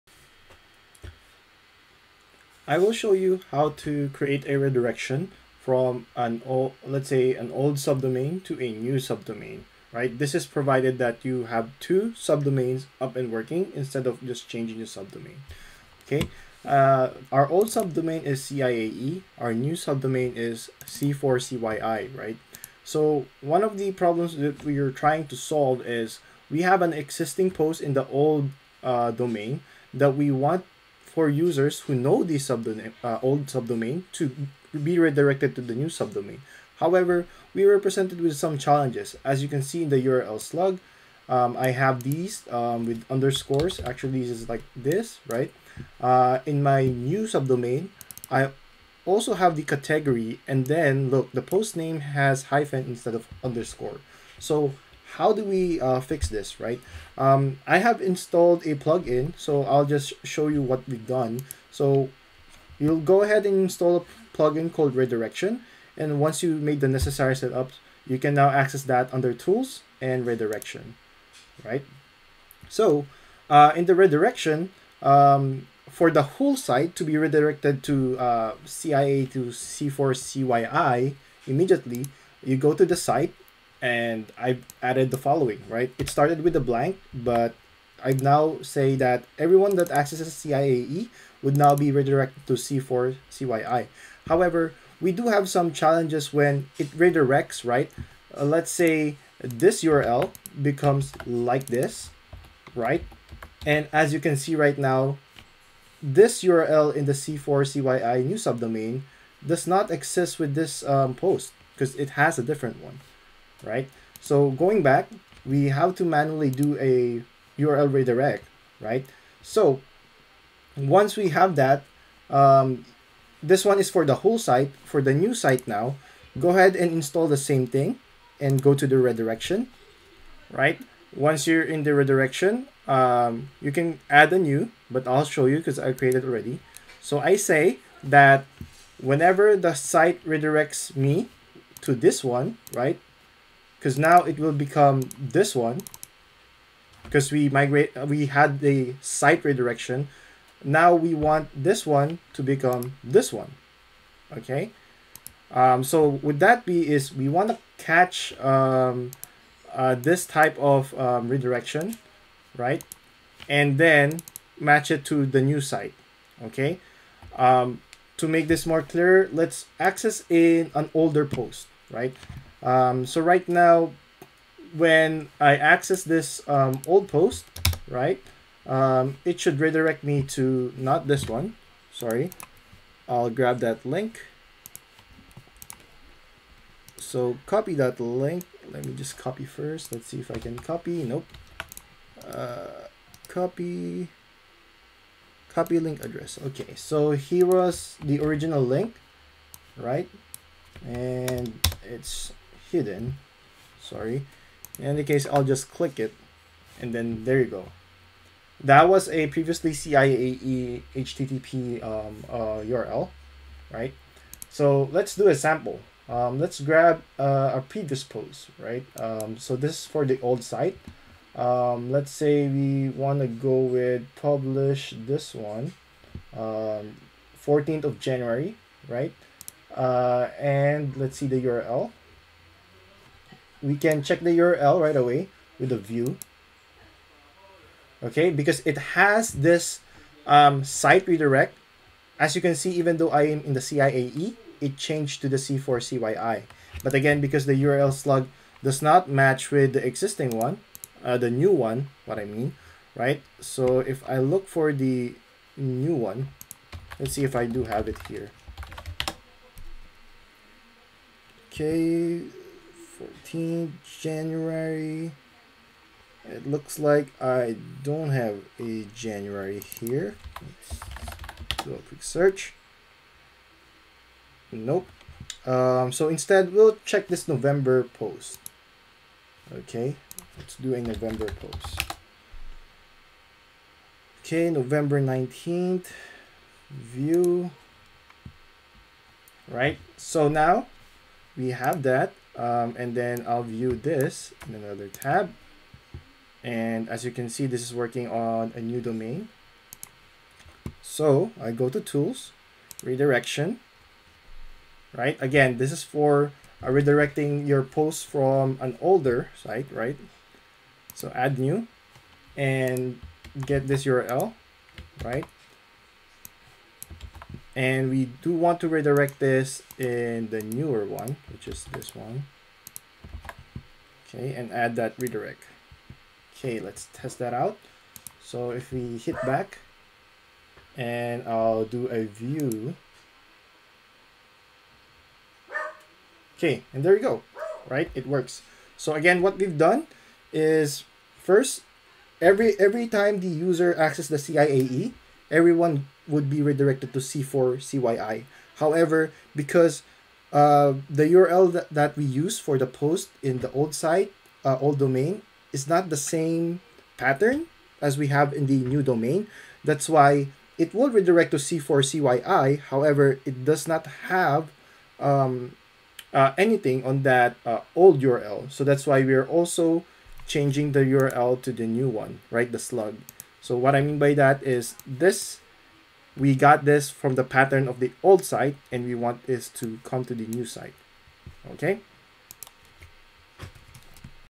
How to Redirect Old Domain to New Domain and Posts with Different URL Slugs
4:40 - Redirect Post from old domain to Post in new domain